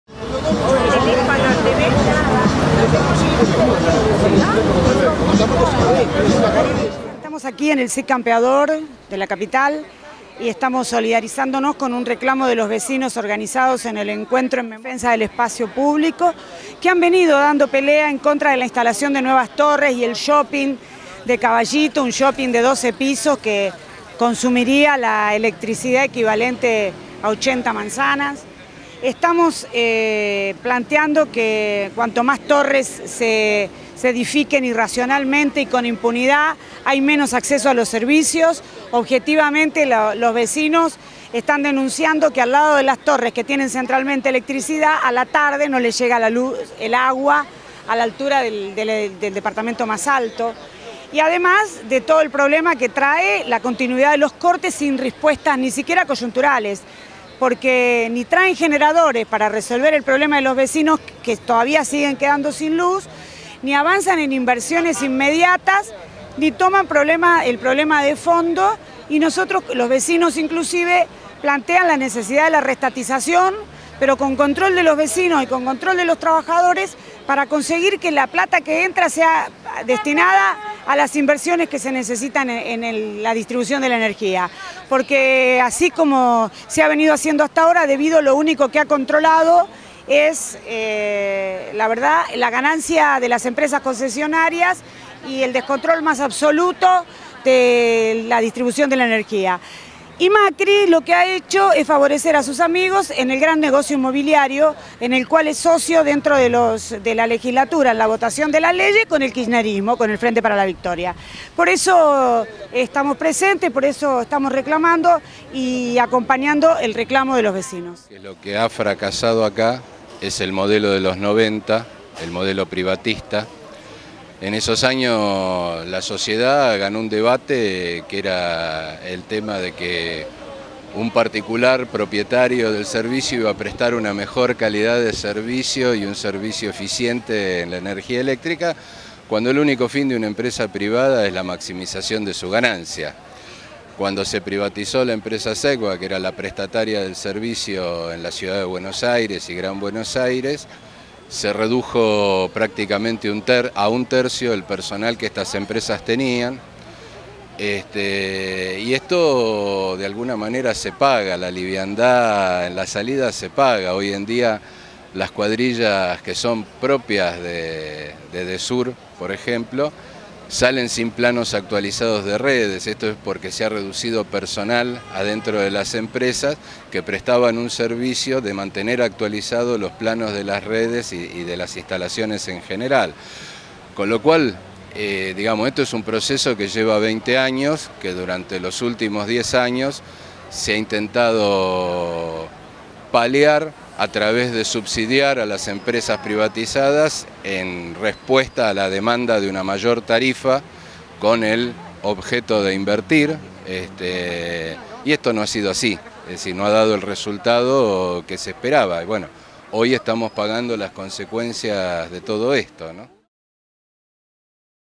Asamblea contra los cortes de luz 21 01 2014
Imágenes de la Asamblea contra los cortes de luz realizada en el Cid Campeador el día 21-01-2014. Entrevistas a Vilma Ripoll, dirigente nacional del MST Nueva Izquierda y Eduardo Bunster, delegado general de ATE - ENRE. Para más información ver